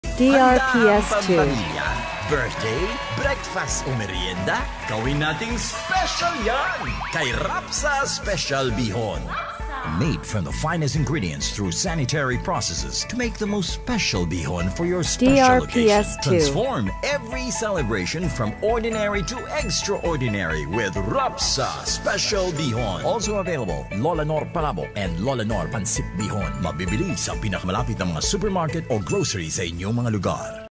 Rapsa Bihon Radio Ad 2023-2024 31s
Ito po ang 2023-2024 Radio Ad ng Rapsa Bihon.

DRPS Two 6504: Rapsa Bihon Radio Ad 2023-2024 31s